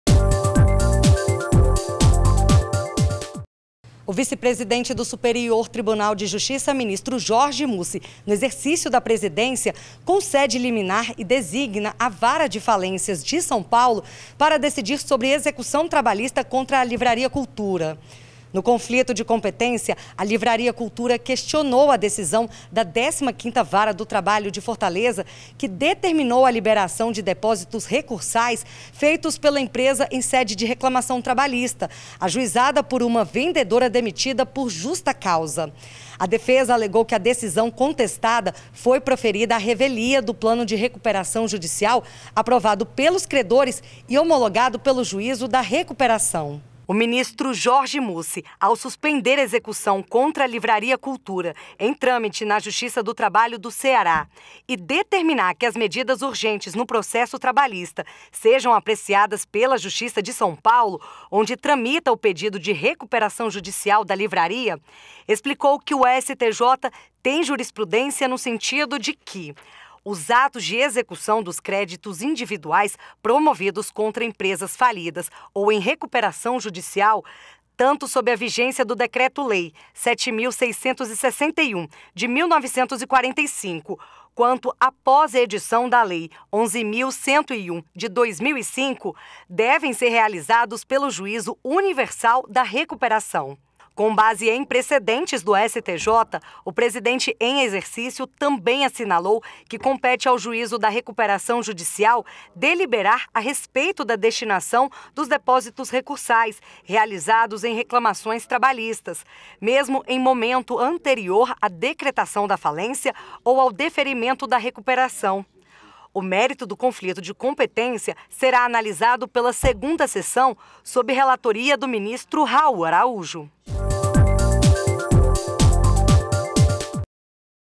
Vara de Falências de São Paulo decidirá sobre execução contra Livraria Cultura 31.01.22
​O vice-presidente do Superior Tribunal de Justiça (STJ), ministro Jorge Mussi, no exercício da presidência, suspendeu execução contra a Livraria Cultura em trâmite na Justiça do Trabalho do Ceará e determinou que as medidas urgentes no processo trabalhista sejam apreciadas pela Justiça de São Paulo, na qual tramita o pedido de recuperação judicial da livraria.
A liminar foi deferida em conflito de competência entre a 2ª Vara de Falências e Recuperações Judiciais do Foro Central de São Paulo e a 15ª Vara do Trabalho de Fortaleza.